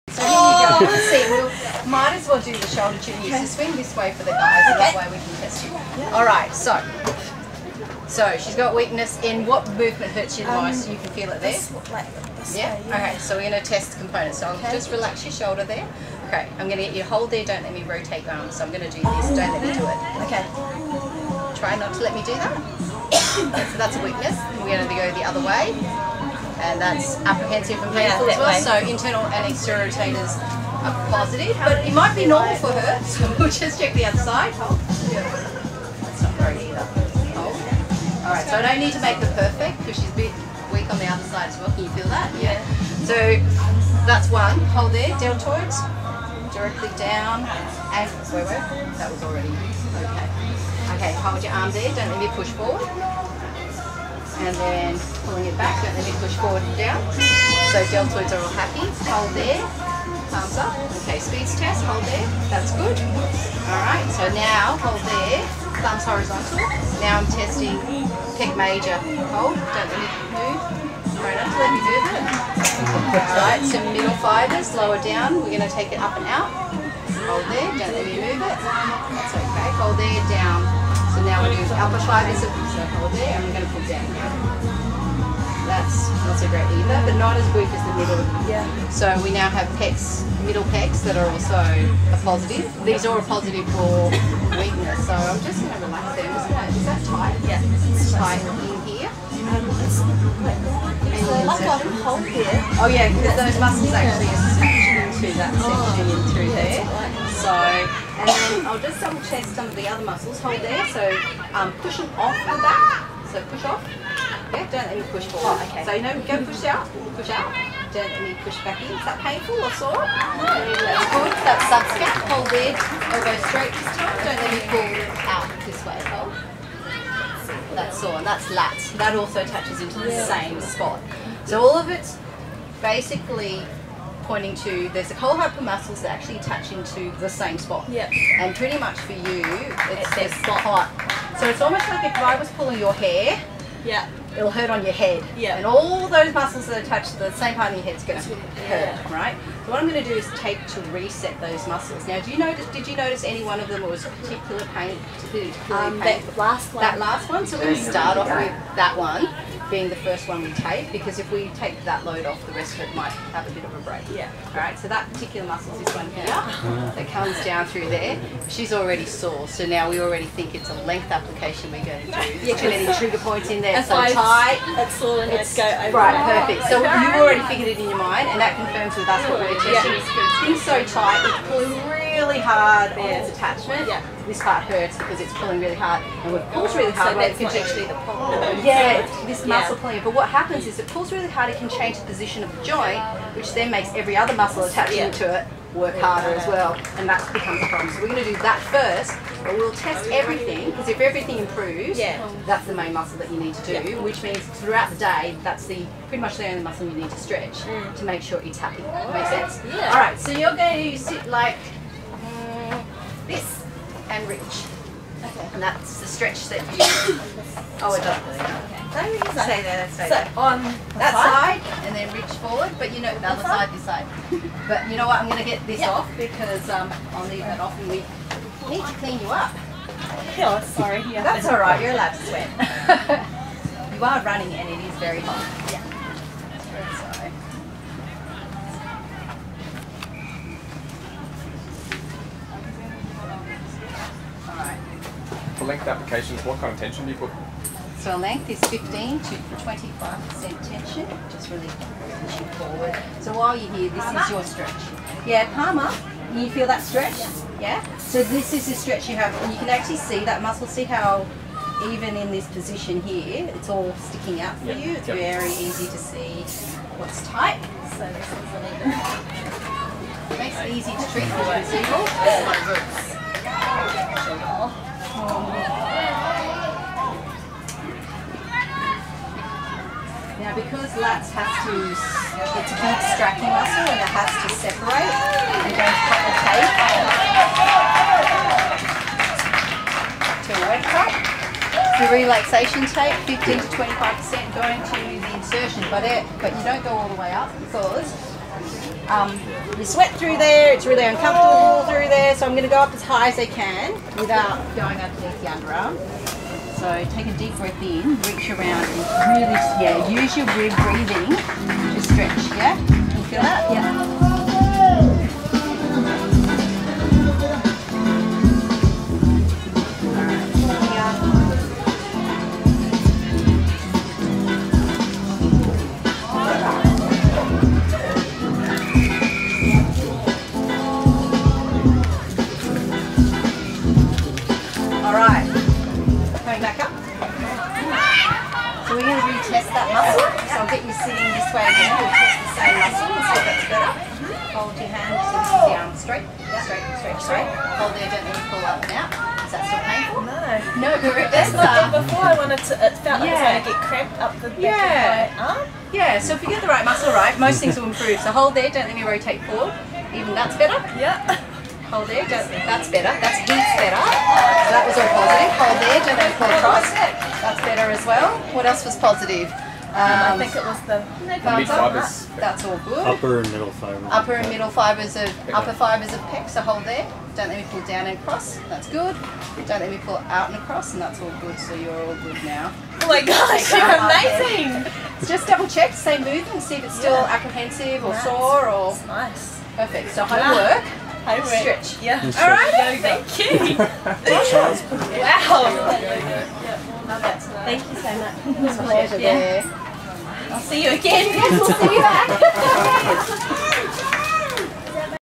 Kinesio Taping Lats to restore range and strength and eliminate shoulder pain
Thuy Bridges testing and taping a player at the Central Coast Sevens International Rugby Tournament (October 2016).

The player presented with pain in the right shoulder and identified movements which exacerbated the condition.
Muscle function testing identified a number of weak muscles (all of which shared a common attachment at the site of pain).
The lats were chosen as the most likely source of the pain and palpation identified this structure to also be very tight.

Kinesio Tape was applied with "relaxation" (inhibition) tension.

Immediately after the taping application, the symptoms were no longer present throughout range and the strength of the previously weak muscle structures was restored.

As you can see, the player was very happy (as well as being pretty amazed by what just happened in less than 7 minutes!).